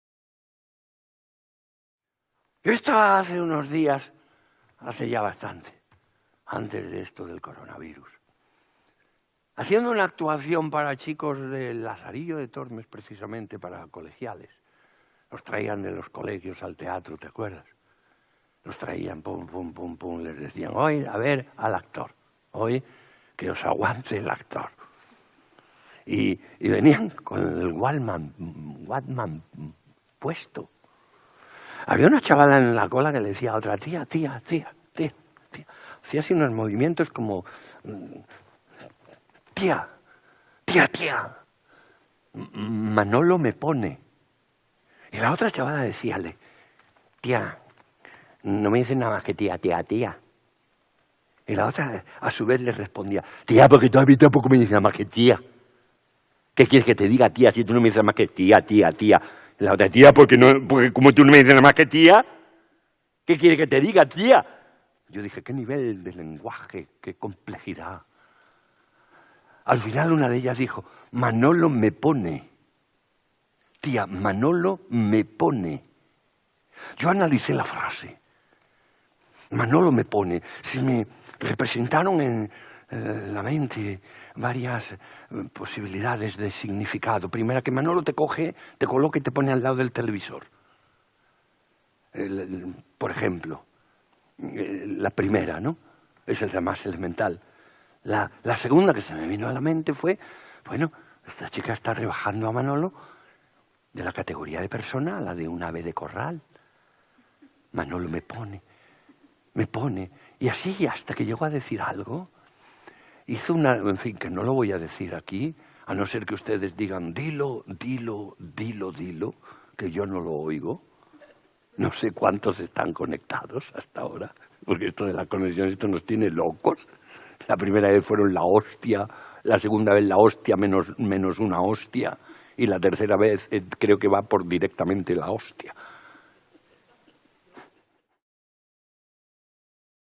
Cómico, un viaje al interior de cada historia
«A mí Jerry Lewis siempre me fascinó... y Groucho Marx… ¡Y Chaplin, sobre todo Chaplin!, porque Chaplin unifica la comedia con el lirismo; con la poesía más tierna, y a veces incluso con una profundidad mística. Tiene una sensibilidad exquisita con un punto de risa.»